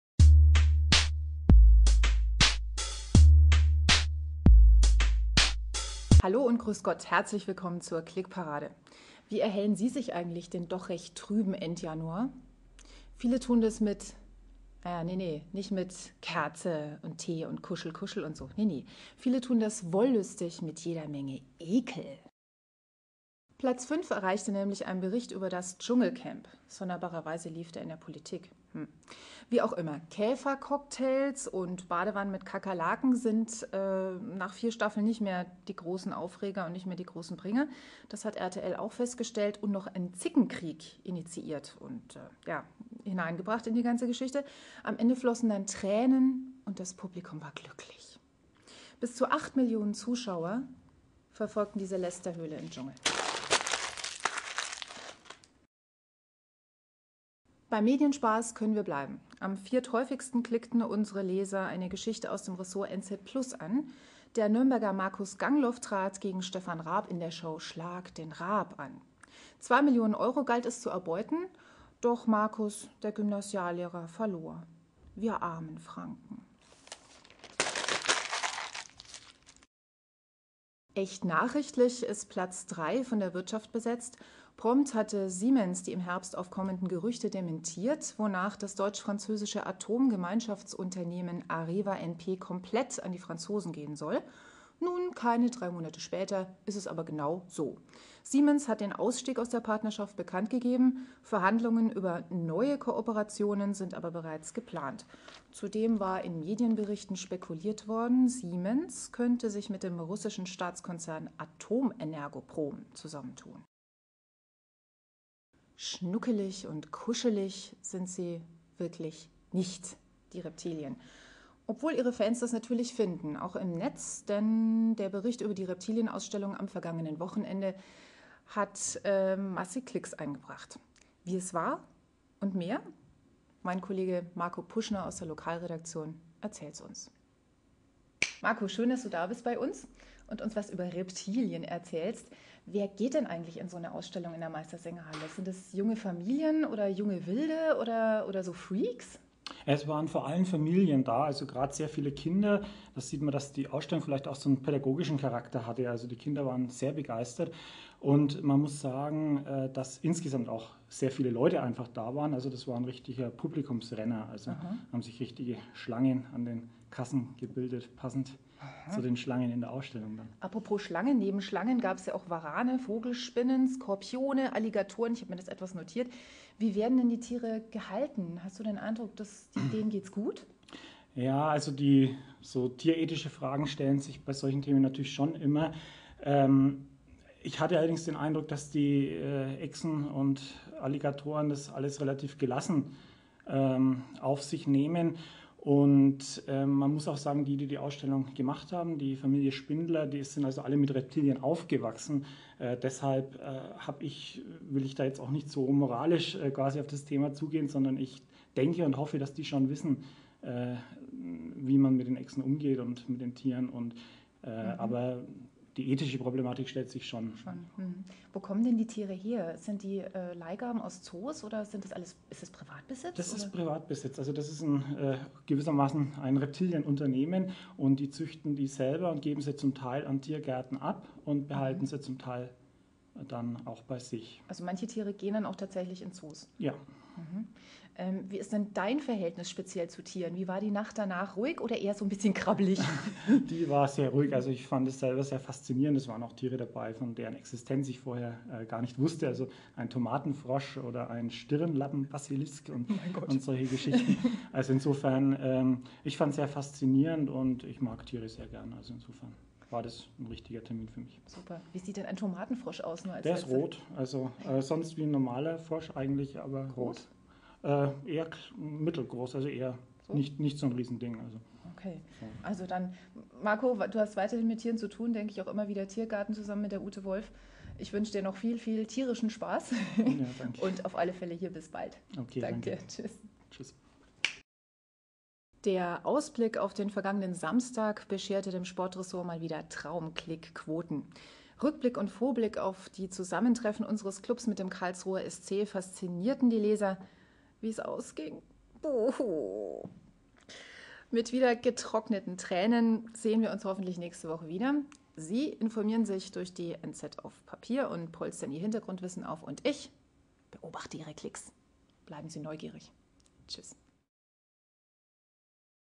Die Klickparade der NZ vom 30.01.2009
Sie wollen wissen was andere NZ-User am meisten interessiert: Die NZ-Klickparade präsentiert die fünf Top-Themen der Woche und wie die Zeitung damit umgeht. Ab Freitag, im Netz. Studiogast: Dr. Marco Puschner. Von Anabel Schaffer.